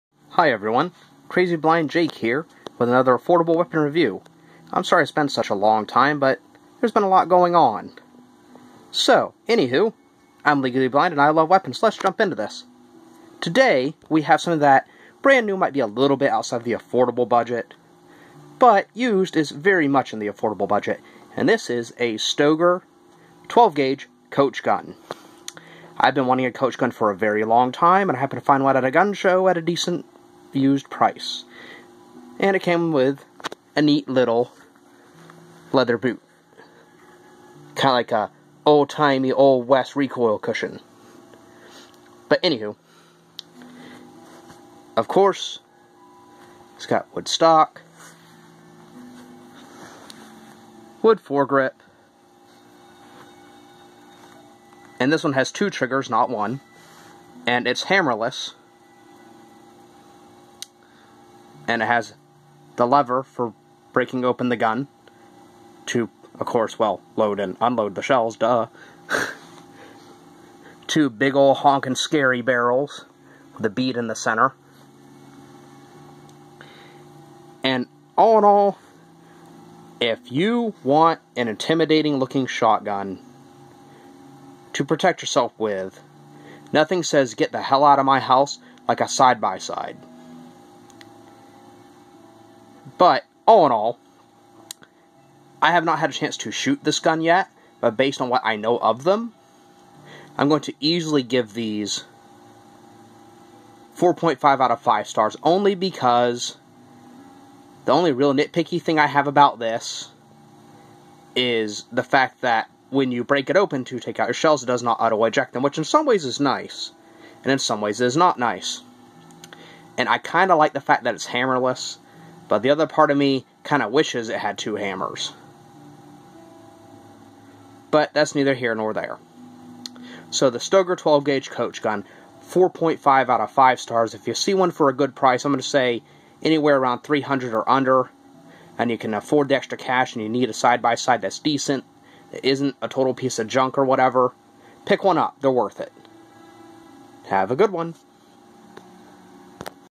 Affordable weapon review. This Is My Boomstick! Stoeger 12 gauge coach gun 4.5 out of 5 stars
Quick review of the Stoeger 12 gauge coach gun. A nice side by side for people who love cowboy action shooting or just plain want and awesome side by side for home defense. I usually give a 4.5 out of 5 stars and intend to keep this gun for a long time.